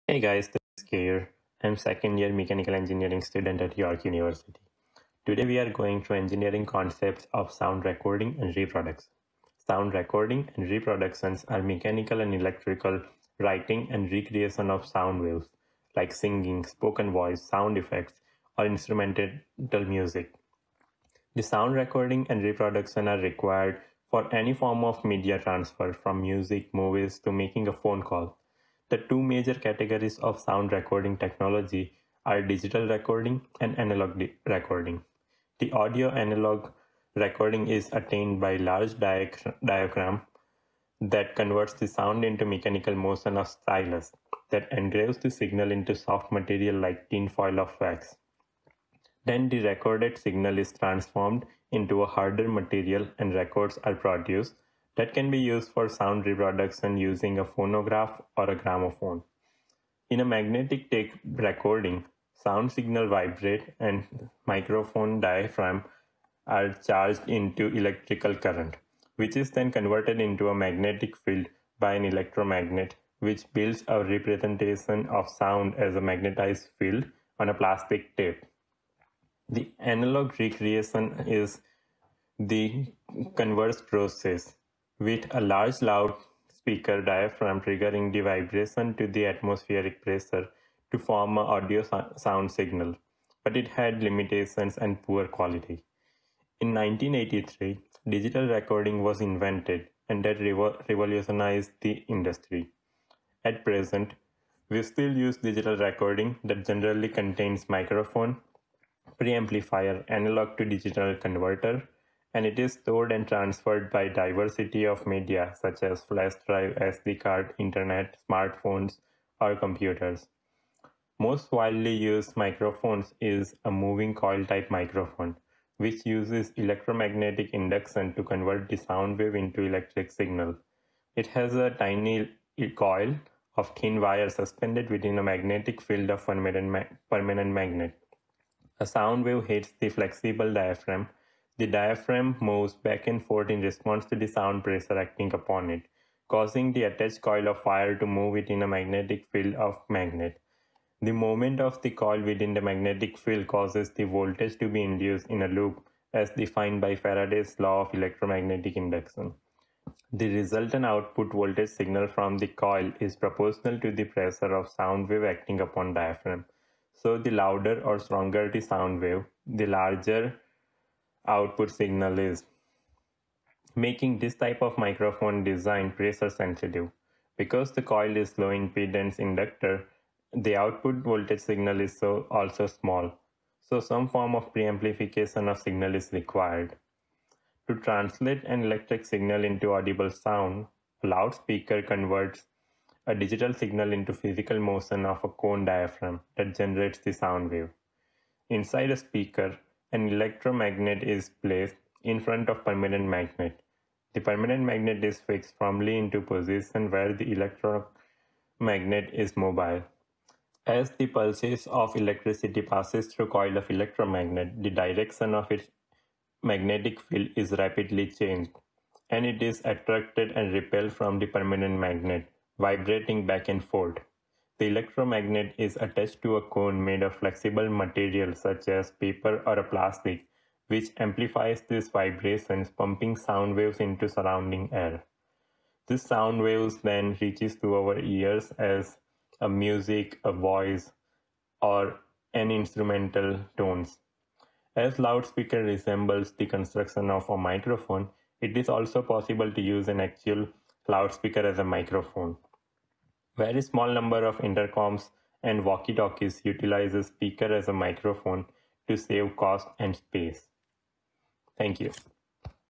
Back-of-Napkin: Sound Recording and Reproduction - Eng 2003
Explanation of Sound Recording and Reproduction engineering concept of virtual napkin within 5 min. Designed and Developed by Keyur Patel - Mechanical Engineering Student at York University as part of Couse Engineering Communications.